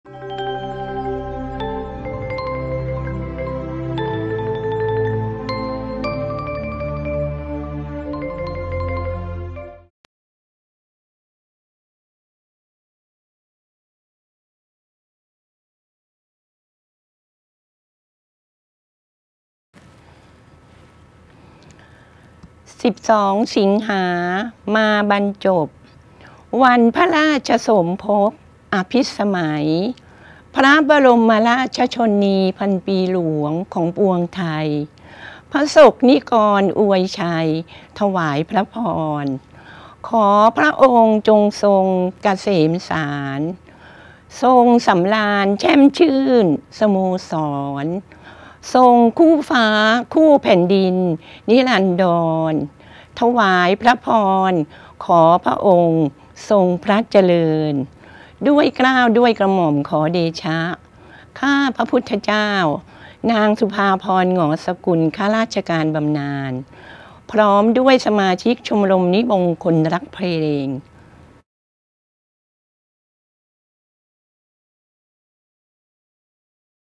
ชมรมนิบงคนรักเพลง
ถวายพระพร สมเด็จพระนางเจ้าสิริกิติ์ พระบรมราชินีนาถ พระบรมราชชนนีพันปีหลวง เนื่องในโอกาสวันเฉลิมพระชนมพรรษา วันที่ ๑๒ สิงหาคม ๒๕๖๗